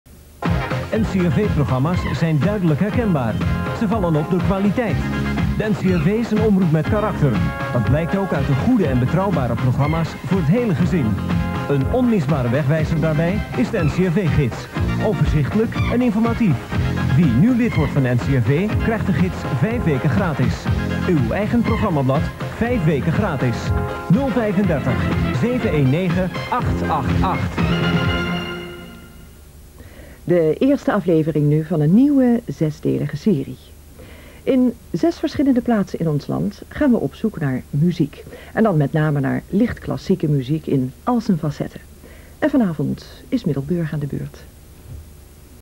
TV: NCRV - NCRV Gids Ledenwerfspot, omroepster Lisette Hordijk (19861004)
NCRV Gids promo en Lisette Hordijk. Uitgezonden op zaterdag 4 oktober 1986.
Met dank aan Jeroen voor het beschikbaar stellen van deze videoband.
Source: Betamax.